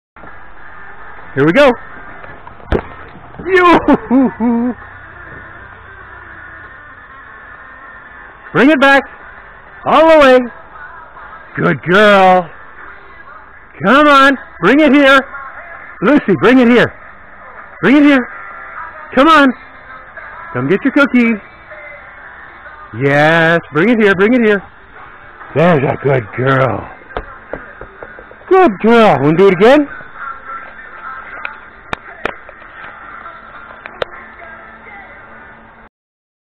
Soccer With Lucy!
Lucy loves chasing the ball and retrieving... sometimes she's not quite so sure where to retrieve it to...   LOL   But she's still the Best Puppy Ever!!